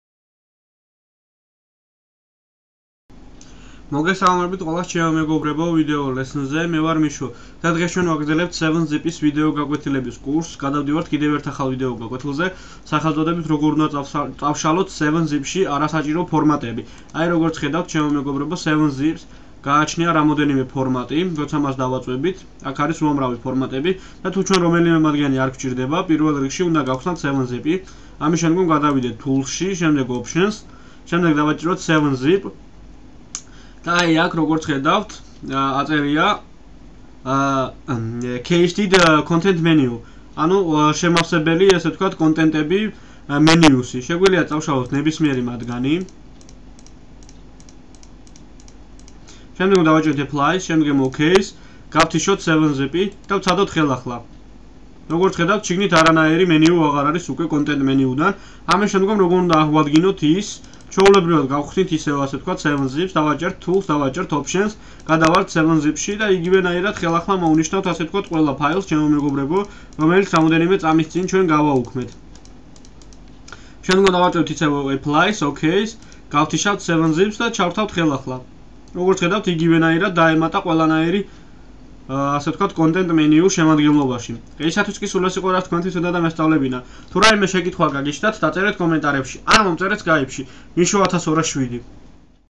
7-Zip-ი დამწყებთათვის (არასაჭირო ფუნქციების ამოღება და ჩასმა)
ამ ვიდეო გაკვეთილების კურსით მე გადავიწყვტე დავიწყო და დაწვრილებით შემოგთავაზოთ ყველაფერი 7-Zip-ის შესახებ, ამ ვიდეო კურსების განმავლობაში ვეცდები დაწვრილებით და სრულყოფილად გასწავლოთ ყველაფერი ამ უფასო და გენიალურ არქივატორზე.